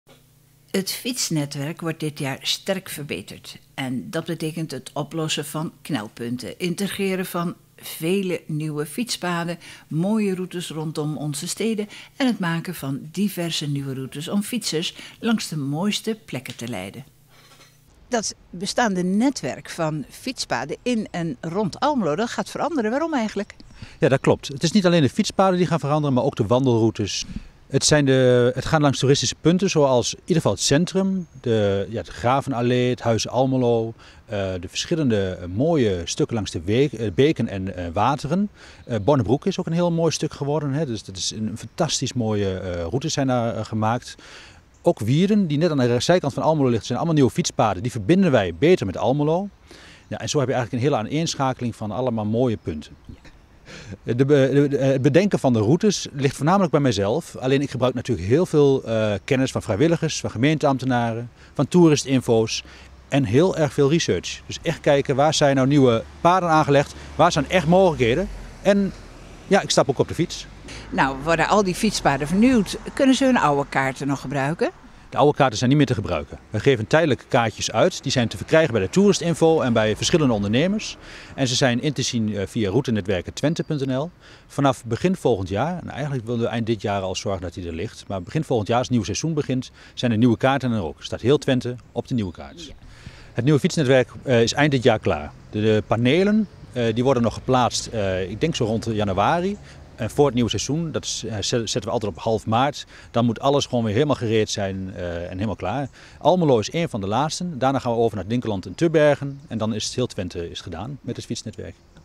Vernieuwing Fietsnetwerk in Almelo
Het bestaande Fietsnetwerk in Twente gaat veranderen. Al 19 jaar dezelfde routes, dan is het tijd voor een update. Nieuwe wegen worden aangelegd en trajecten worden herondekt. Naast de fietsroutes worden de aangelegde wandelroutes in de toekomst geüpdatet.

Een groot deel van de routes in Twente is al vernieuwd, binnenkort starten de werkzaamheden in Almelo. Daarna komen de gemeenten Dinkelland en Tubbergen als laatste in de regio.